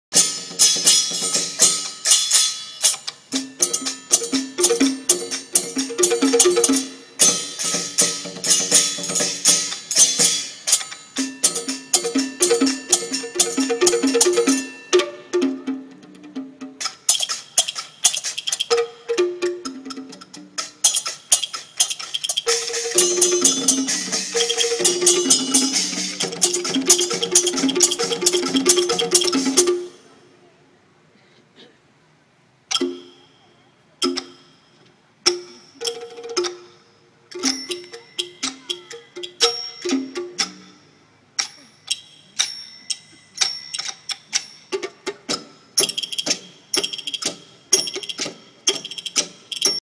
Santiago a Mil: El Encanto del Rio Amarillo: 7
Part of the Santiago a Mil 2012 international theater festival. La Boda del Raton.